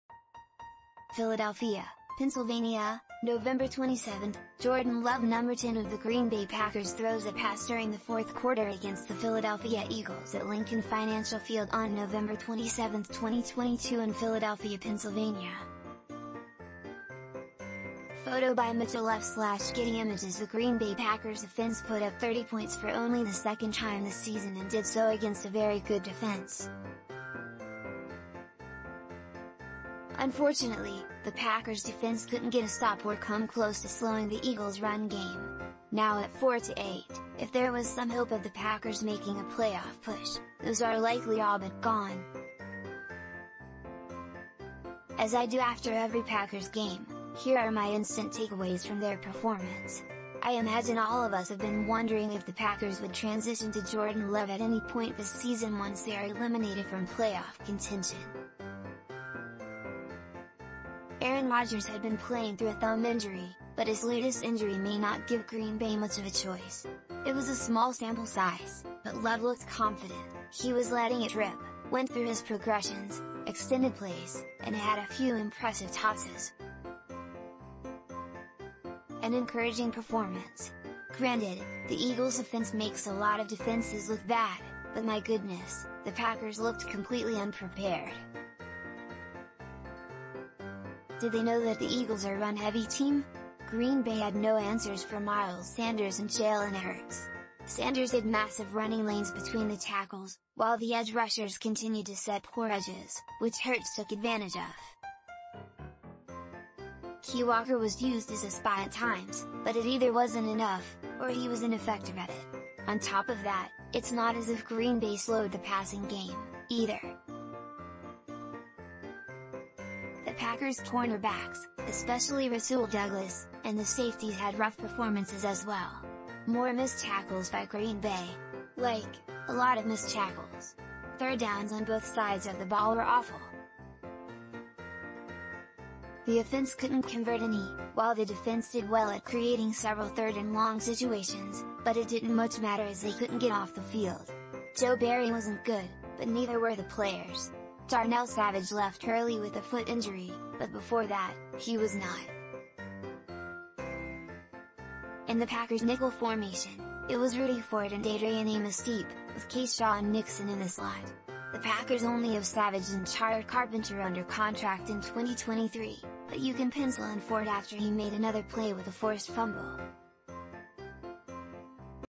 Packers Instant Takeaways Jordan Love takes over for injured Aaron Rodgers
PHILADELPHIA, PENNSYLVANIA - NOVEMBER 27: Jordan Love #10 of the Green Bay Packers throws a pass during the fourth quarter against the Philadelphia Eagles at Lincoln Financial Field on November 27, 2022 in Philadelphia, Pennsylvania. (Photo by Mitchell Leff/Getty Images)The Green Bay Packers offense put up 30 points for only the second time this season and did so against a very good defense. Unfortunately, the Packers defense couldn’t get a stop or come close to slowing the Eagles’ run game. Now at 4-8, if there was some hope of the Packers making a playoff push, those are likely all but gone. As I do after every Packers game, here are my instant takeaways from their performance.– I imagine all of us have been wondering if the Packers would transition to Jordan Love at any point this season once they are eliminated from playoff contention. Aaron Rodgers had been playing through a thumb injury, but his latest injury may not give Green Bay much of a choice.– It was a small sample size, but Love looked confident — he was letting it rip — went through his progressions, extended plays, and had a few impressive tosses. An encouraging performance.– Granted, the Eagles’ offense makes a lot of defenses look bad, but my goodness, the Packers looked completely unprepared. Did they know that the Eagles are a run-heavy team? Green Bay had no answers for Miles Sanders and Jalen Hurts. Sanders had massive running lanes between the tackles, while the edge rushers continued to set poor edges, which Hurts took advantage of. Quay Walker was used as a spy at times, but it either wasn’t enough, or he was ineffective at it.– On top of that, it’s not as if Green Bay slowed the passing game, either. The Packers’ cornerbacks, especially Rasul Douglas, and the safeties had rough performances as well.– More missed tackles by Green Bay. Like, a lot of missed tackles.– Third downs on both sides of the ball were awful. The offense couldn’t convert any, while the defense did well at creating several third and long situations, but it didn’t much matter as they couldn’t get off the field.– Joe Barry wasn’t good, but neither were the players.– Darnell Savage left early with a foot injury, but before that, he was not among the starters. In the Packers’ nickel formation, it was Rudy Ford and Adrian Amos deep, with Keisean Nixon in the slot.– The Packers only have Savage and Tariq Carpenter under contract in 2023, but you can pencil in Ford after he made another play with a forced fumble. He was brought in to be a special teams player, but he has been a solid defender as well.– During the first six minutes of the game, the Packers’ offense, defense, and special teams all looked completely lost. Credit to Green Bay for continuing to fight and making this game competitive. It could have been very easy to just give in.– Justin Hollins, who was claimed off waivers on Wednesday, had a nice debut with a run stop, a sack, and a pressure.